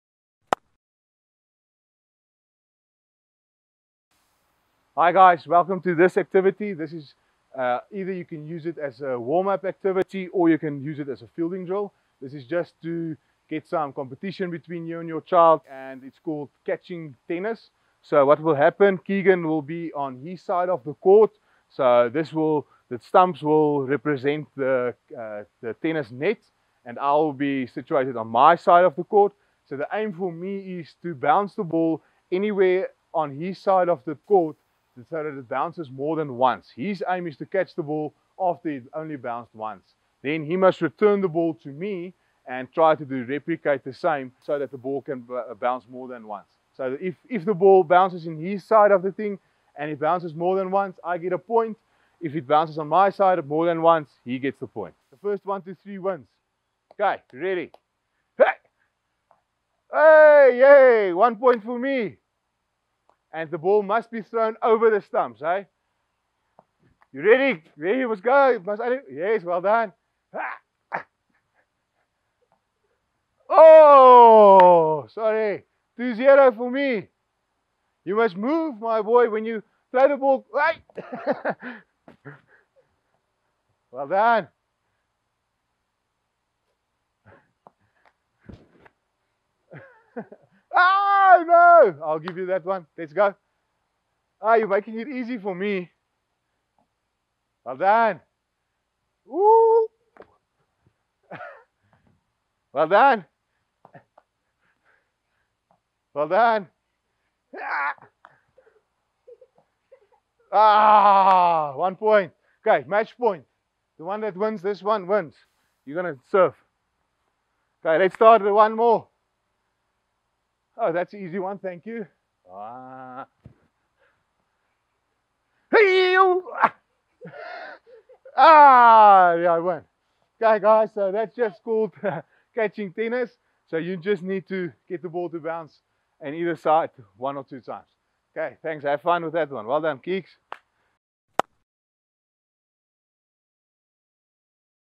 Cricket Kiddo | Catching Tennis | E07
In this seventh episode of Cricket Kiddo we go through this interesting fielding drill called catching tennis. Watch to find out more.

If you enjoyed our content be sure to follow us on the channels below for even more inspiring and enjoyable content.